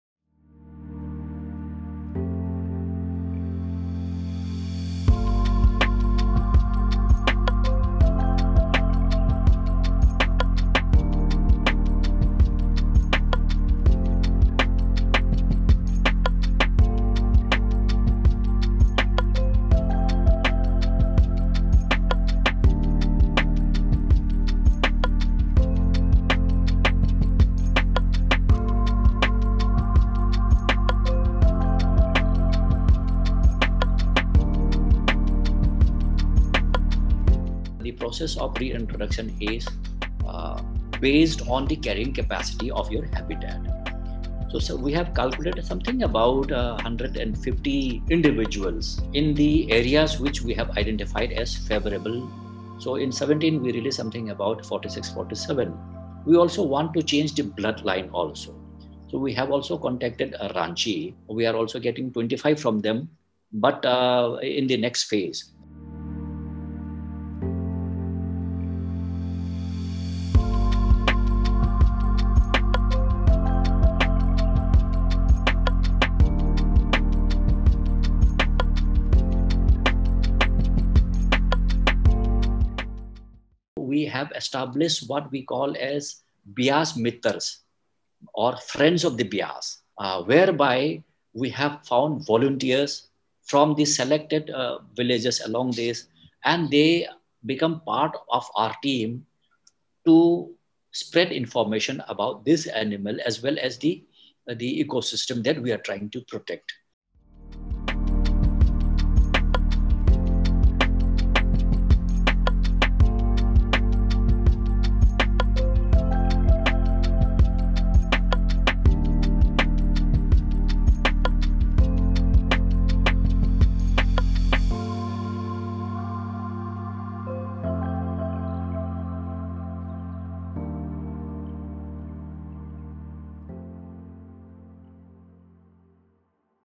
Gharials are back in Punjab's river. But the real test is breeding.
The critically endangered gharials had gone locally extinct in Punjab's rivers decades ago. A reintroduction programme has brought back the reptiles to the Beas river.

Forty five of the 47 gharials brought from Madhya Pradesh’s Morena captive breeding centre to Punjab about three years ago have been rehabilitated into the Beas river. More gharials are set to be released in the river from other breeding centres across the country.

The Punjab gharials are now sub-adults and the next crucial phase is to see if they are able to breed and multiply in years to come. Females gharials generally lay eggs when they are 10 to 12 years of age while signs of male adulthood include developing a round bump at the tip of their snout that looks like a 'ghara' (Hindi word for earthen pot).